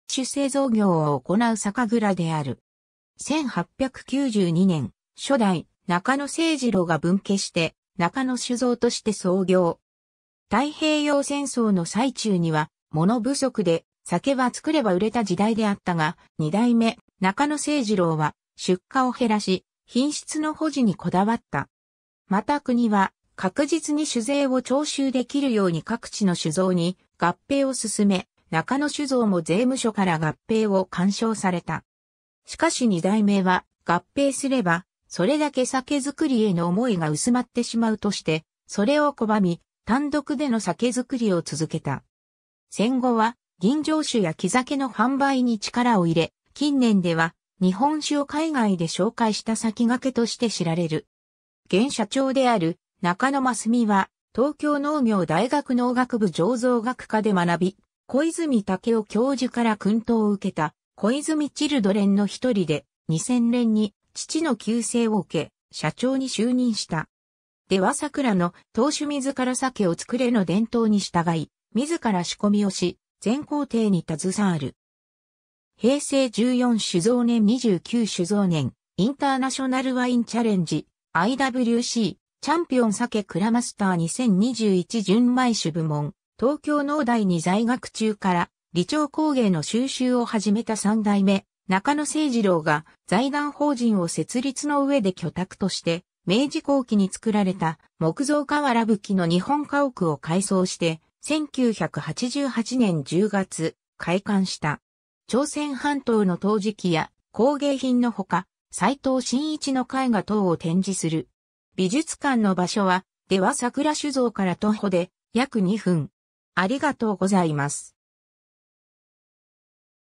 出羽桜酒造
出羽桜酒造株式会社（でわざくらしゅぞう）は、山形県の清酒製造業を行う酒蔵である。
 1892年、初代仲野清次郎が分家して仲野酒造として創業。
 太平洋戦争の最中には、物不足で酒は造れば売れた時代であったが、2代目仲野清次郎は出荷を減らし、品質の保持にこだわった。
また国は確実に酒税を徴収できるように各地の酒造に合併を進め、仲野酒造も税務署から合併を勧奨された。
しかし2代目は「合併すれば、それだけ酒造りへの思いが薄まってしまう」としてそれを拒み、単独での酒造りを続けた。
 戦後は、吟醸酒や生酒の販売に力を入れ、近年では日本酒を海外で紹介したさきがけとして知られる。
 現社長である仲野益美は、東京農業大学農学部醸造学科で学び、小泉武夫教授から薫陶を受けた「小泉チルドレン」の一人で、2000年に父の急逝を受け、社長に就任した。
出羽桜の「当主自ら酒を作れ」の伝統に従い、自ら仕込みをし全行程に携わる。
 平成14酒造年 - 29酒造年 インターナショナル・ワイン・チャレンジ（IWC)「チャンピオン・サケ」 クラマスター2021純米酒部門 東京農大に在学中から李朝工芸の蒐集を始めた3代目仲野清次郎が、財団法人を設立の上で居宅として明治後期に造られた木造瓦ぶきの日本家屋を改装して、1988年10月開館した。朝鮮半島の陶磁器や工芸品のほか、斎藤真一の絵画等を展示する。 美術館の場所は、出羽桜酒造から徒歩で約２分｡